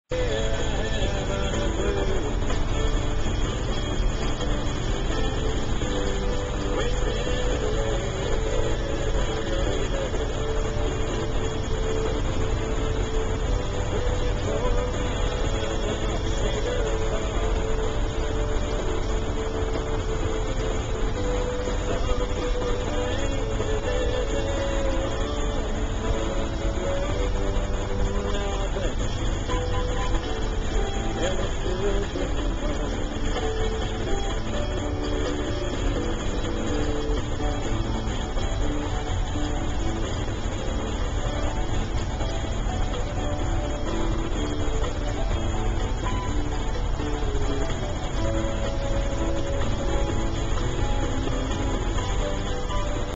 Falling Asleep
Here's Elliott in the truck falling asleep